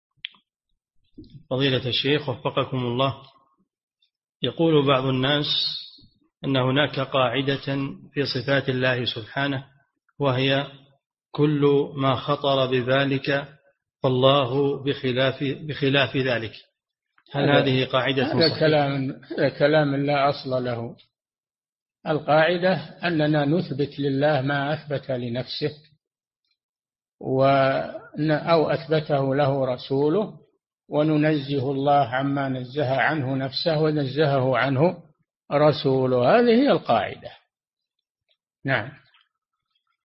[79 -308] ما صحة هذه القاعدة في أسماء الله وصفاته: كل ما خطر ببالك فالله بخلاف ذلك؟ - صالح الفوزان
-[79 -308] السؤال: يقول بعض الناس إن هناك قاعدةً لصفات الله سبحانه، وهي: كل ما خطر ببالك فالله بخلاف ذلك، هل هذه قاعدةٌ صحيحة؟
الجواب: هذا كلام لا أصل له، القاعدة: أننا نُثبت لله ما أثبت لنفسه، أو أثبته له رسوله، ونُنزه الله عما نزه عنه نفسه أو نزهه عنه رسوله، هذه هي القاعدة.
للاشتراك في قناة الشيخ صالح الفوزان

للاستفسار والإقتراحات والدعم
رئيس الفريق
لمشروع كبار العلماء